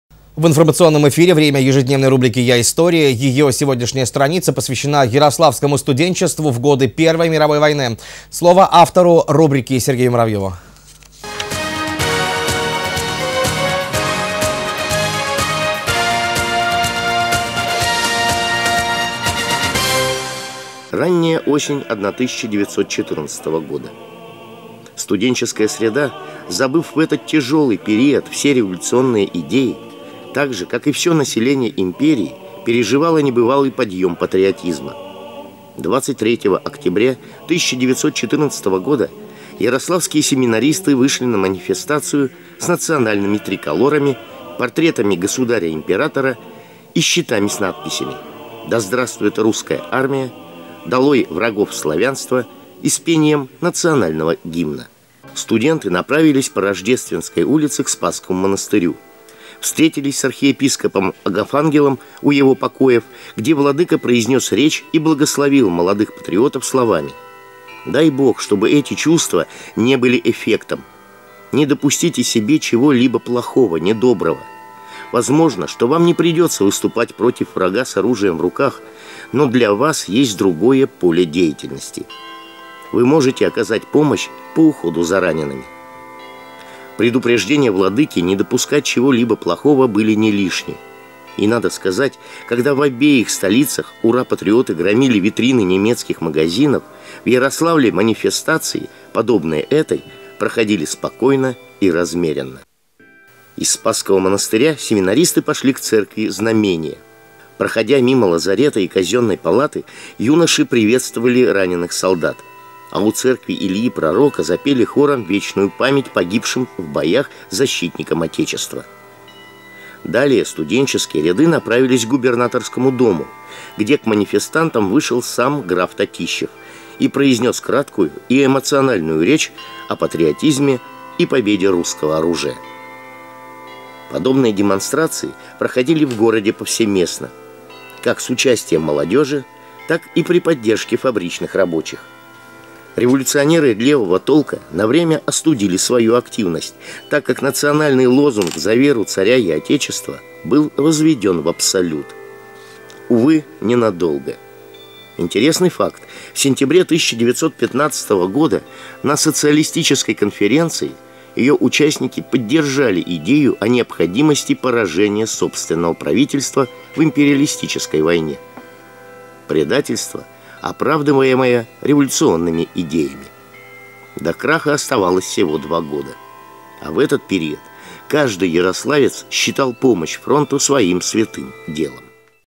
РУБРИКА Я — история: Ярославское студенчество в годы
РУБРИКА Я — история: Ярославское студенчество в годы Первой мировой

Её сегодняшняя страница посвящена ярославскому студенчеству в годы Первой мировой войны.